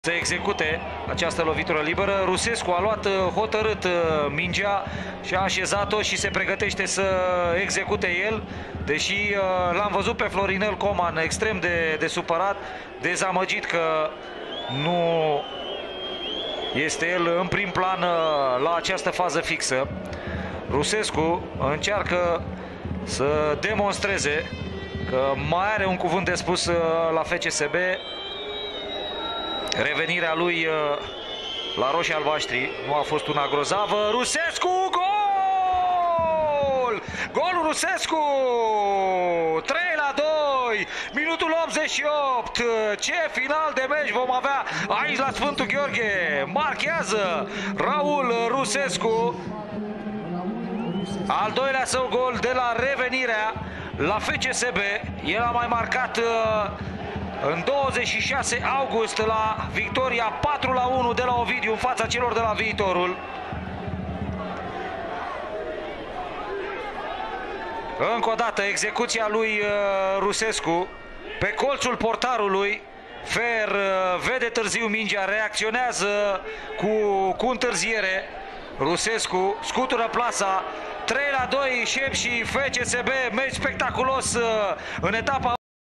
Sepsi OSK - FCSB: 3-2 gol Rusescu din lovitura libera (87) Liga 1
Au marcat Vasvari, Stefan, Tandia / Coman, Rusescu

Echipele de start:

Sepsi: Fejer - Moura, Viera, Jovanovici, Sato - Velev, Rus - Nouvier, Vașvari, Fl. Ștefan - Tandia
Rezerve: Niczuly, Ursu, Karnitsky, Mensah, Hamed, Drăghici, Simonovski
Antrenor: Eugen Neagoe

FCSB: Bălgrădean - Benzar, Planici, Bălașa, Morais - Nedelcu, Filip - Teixeira, Man, Fl. Tănase - Gnohere
Rezerve: Vlad, Fl. Coman, Jakolis, I. Stoica, M. Roman, R. Rusescu, Moldovan
Antrenor: Nicolae Dică

Arbitru: Horațiu Feșnic / Asistenți: Alexandru Cerei și Mircea Orbuleț Urmareste stirile zilei din sport pe pagina COMUNITATII FOTBAL ROMANIA